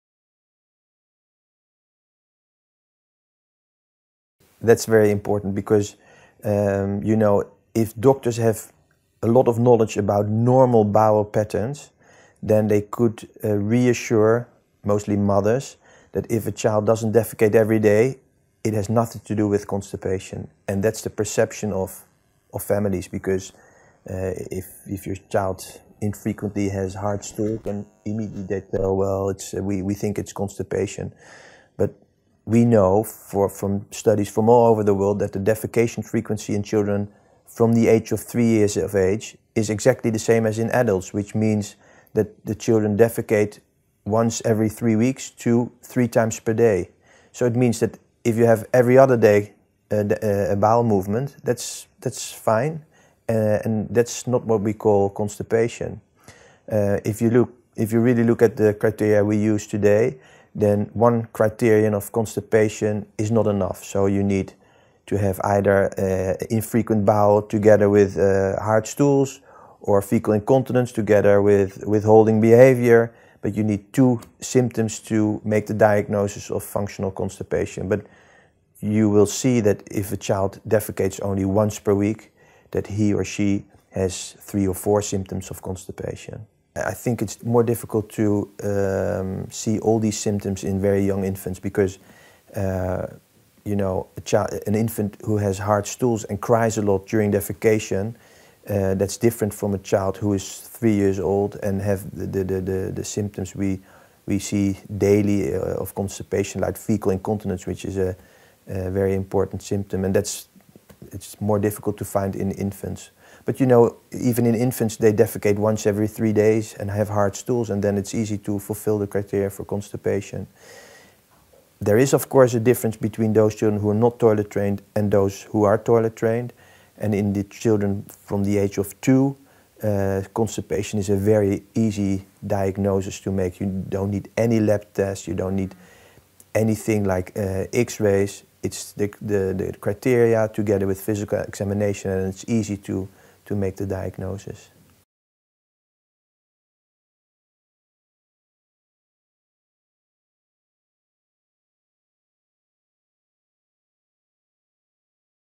Interview Marc Benninga - Gejala yang Dikenali Secara Klinis Dibandingkan dengan Persepsi Orangtua
FrieslandCampina Institute. Interview Marc Benninga - Gejala yang Dikenali Secara Klinis Dibandingkan dengan Persepsi Orangtua.